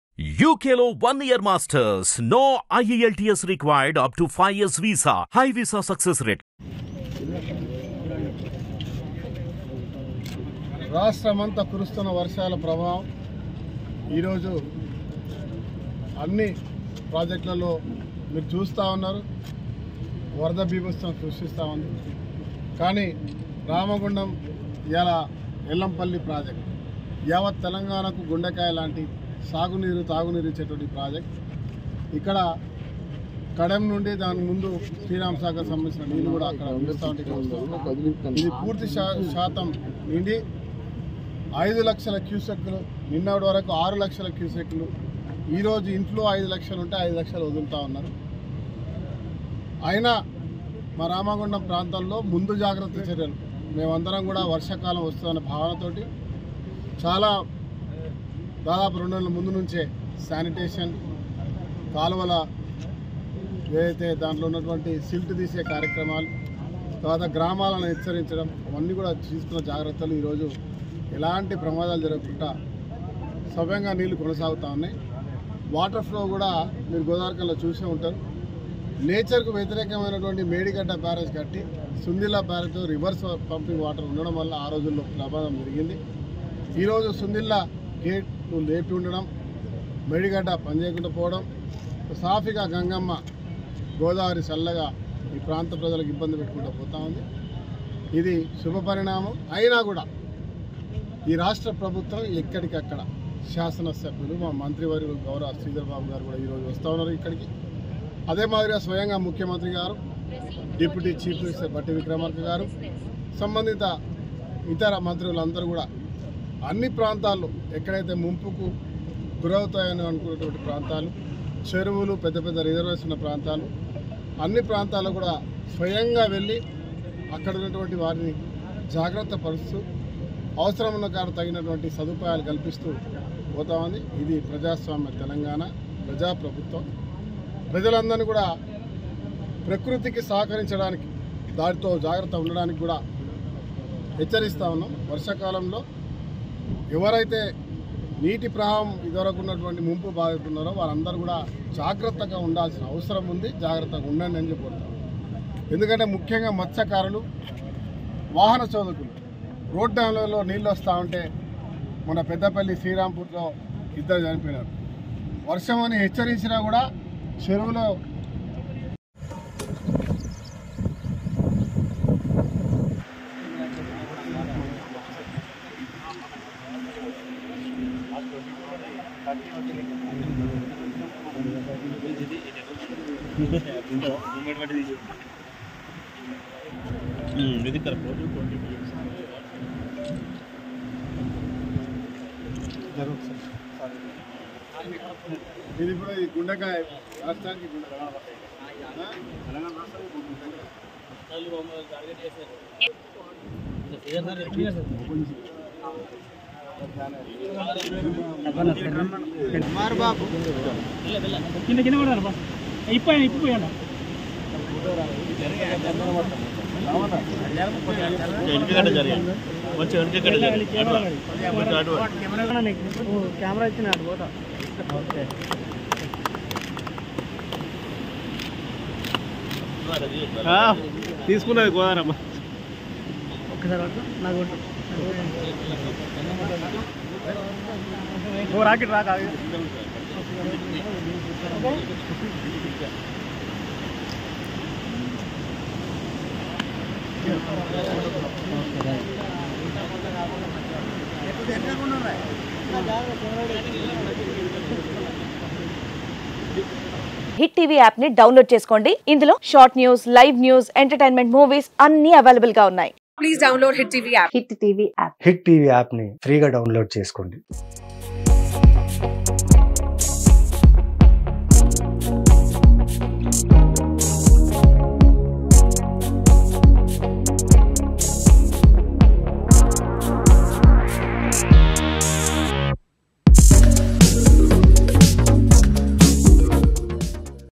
ఎల్లంపల్లి ముంపు ప్రాంతాలలో పర్యటించిన ఎమ్మెల్యే రాజ్ ఠాకూర్ | Ellampalli Project
Watch : ఎల్లంపల్లి ముంపు ప్రాంతాలలో  పర్యటించిన ఎమ్మెల్యే రాజ్ ఠాకూర్ | Ellampalli Project  @Hittvtelugunews